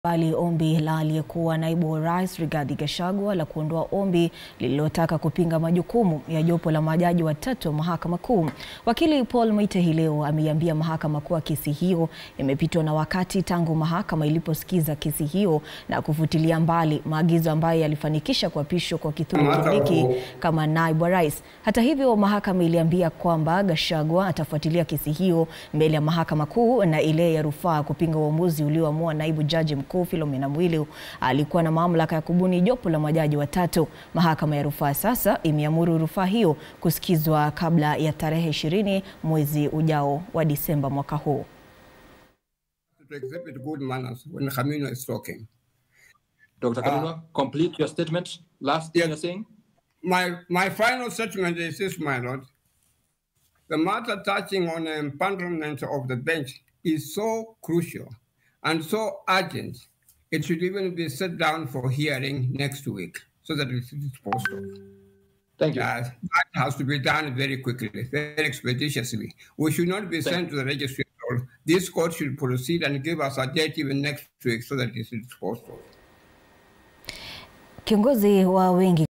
Gachagua aondoa ombi la kupinga jopo la majaji watatu
Mahakama ya rufaa imekubali ombi la aliyekuwa Naibu wa Rais Rigathi Gachagua la kuondoa ombi lililotaka kupinga majukumu ya jopo la majaji watatu wa mahakama kuu.